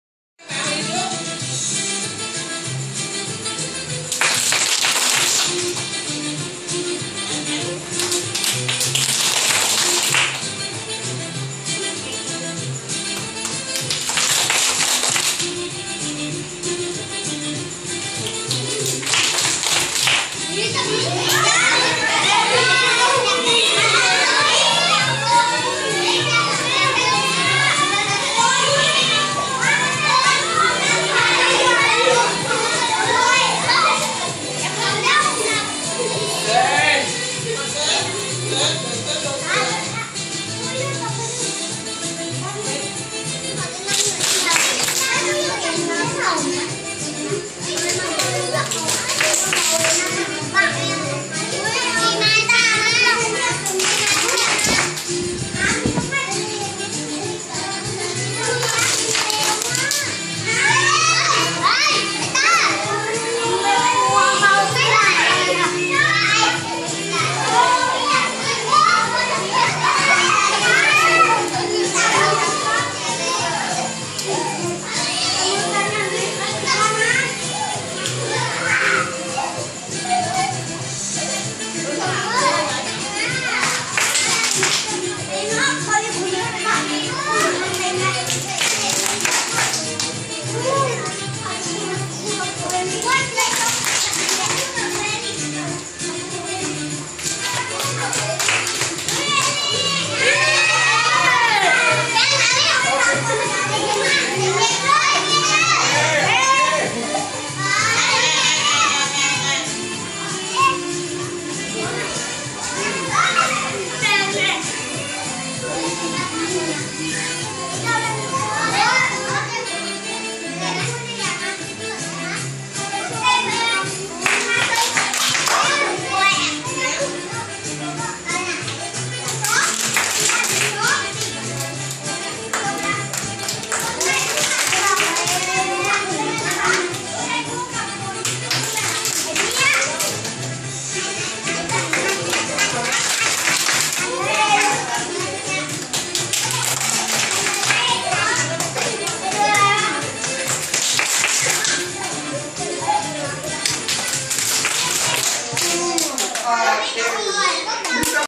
สนุกมากหลังเรียนเสร็จ l เด็กๆ อ.2 ภาษาอังกฤษกับครูต่างชาติ
After finished class
จัดการเรียนการสอนโดยครูชาวต่างชาติ Teacher Eric โรงเรียนชุมชนวัดย่านขาด(ประชาสงเคราะห์) อ.พรหมพิราม สังกัดสำนักงานเขตพื้นที่การศึกษาประถมศึกษาพิษณุโลก เขต3 บูรณาการการจัดการเรียนรู้เข้ากับเทคเนิคการสอนแบบ CLT(Communicative Language Teaching) ภาคเรียนที่1 ปีการศึกษา 2558